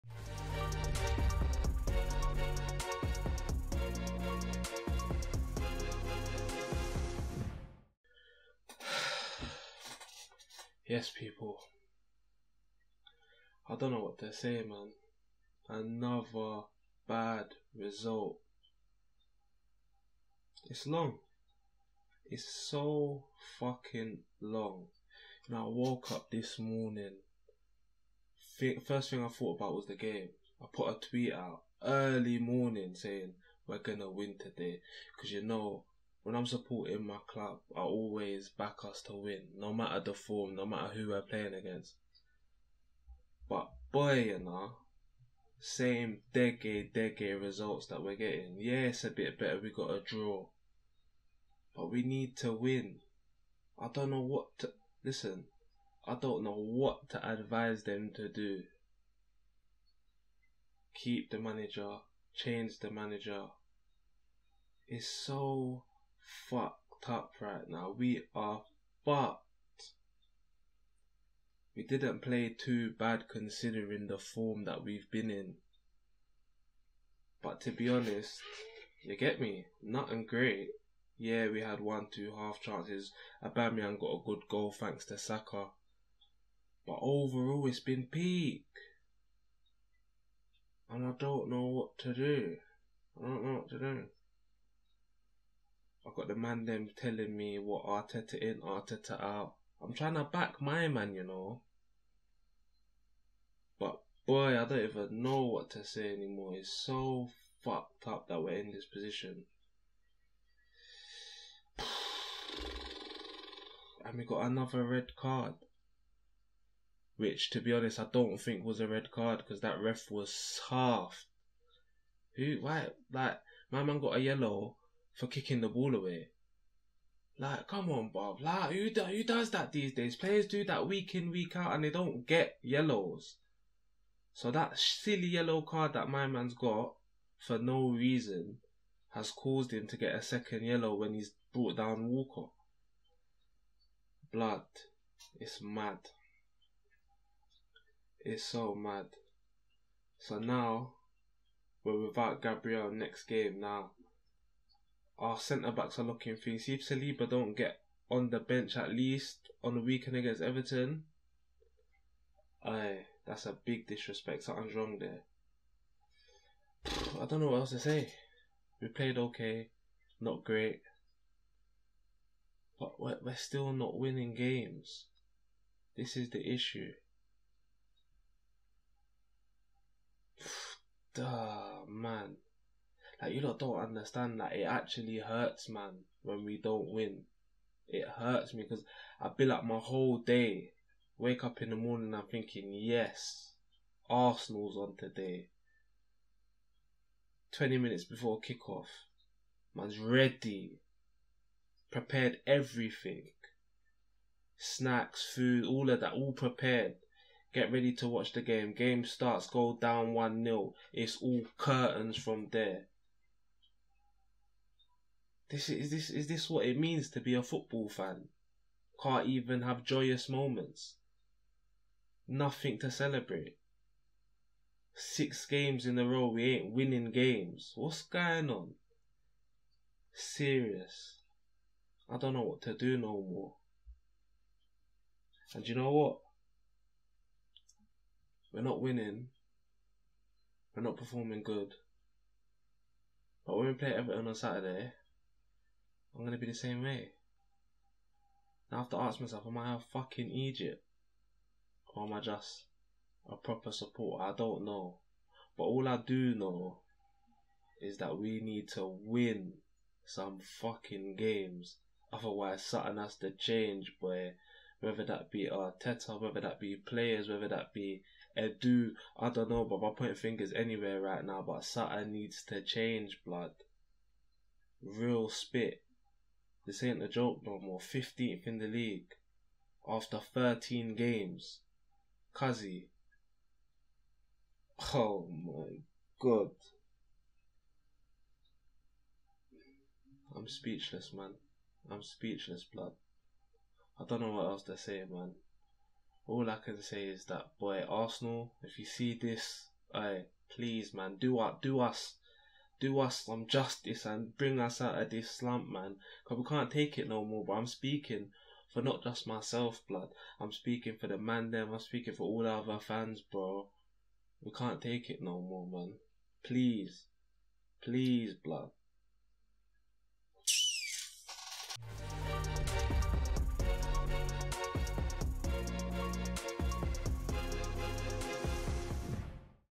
Arsenal 1-1 Southampton - MORE POINTS DROPPED! WHAT NEXT? | ARSENAL FC MATCH REACTION
More points dropped today against Southampton. Where do we go from here?